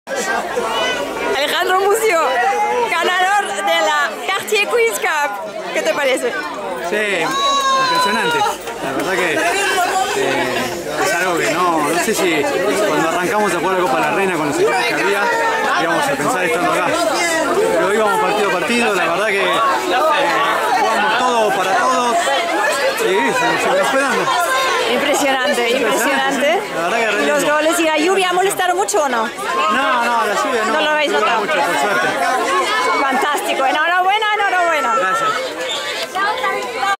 NG polo TV Interview Ale Muzzio, MURUS SANCTOS, Ganador CARTIER Queen¨s Cup 2023. Guards Polo Club
ICONIC Cartier Queens Cup, UK, high goal Polo - Guards Polo Club, Windsor. 17 world¨s finest Teams - 3 semanas - 43 partidos - 68 Player CRACKs - 1 ganador: MURUS SANCTUS
Corinne Ricard, Polito Pieres, Ale Muzzio, Marcus Araya

Final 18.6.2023: Murus Santus  11. vs. 10 La Magdelaine.